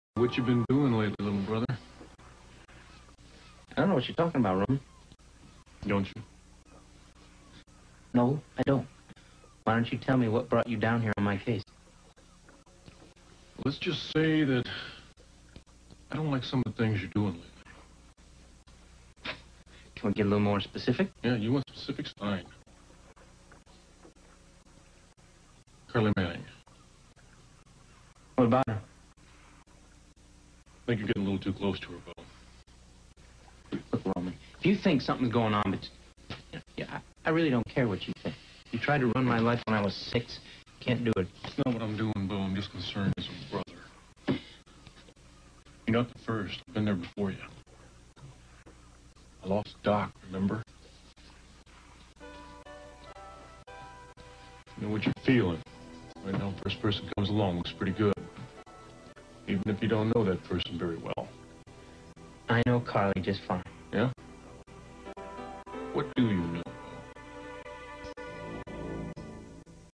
Missm.: Back From New York Part 3 1990 (0-1:09)
I just jumped back to some earlier episodes because missmonroe78 has added some more clips from this time period. So I'm piecing together some earlier clips and then I'll resume to the later eps.
Bo dodges Roman's questions.